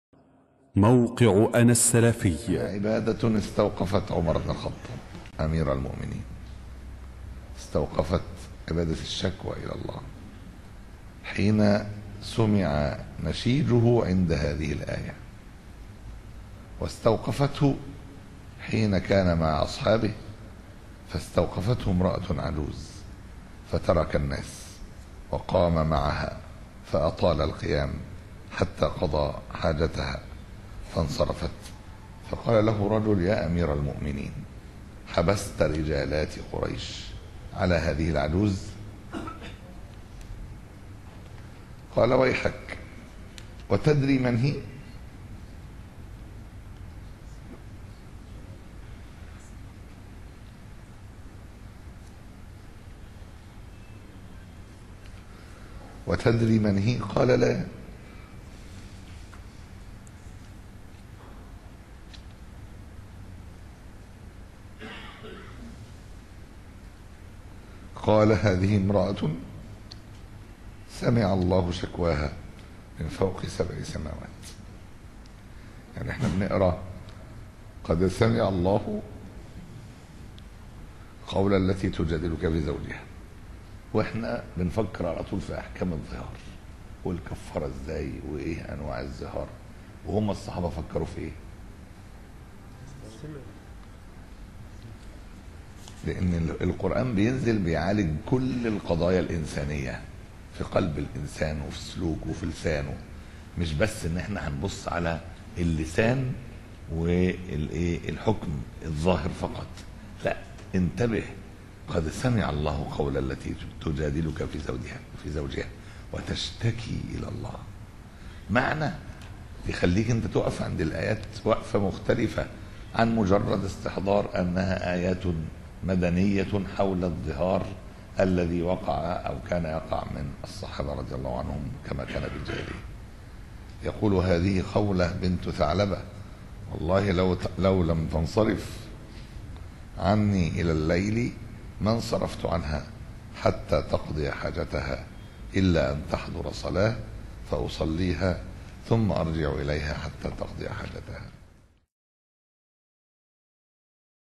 لذة الشكوى إلى الله (مقطع). د/ ياسر برهامي
مقطع من مادة بعنوان: 027- كرب جديد فوق الحزن- الآيات (83 - 86) (الداعية في كل مكان)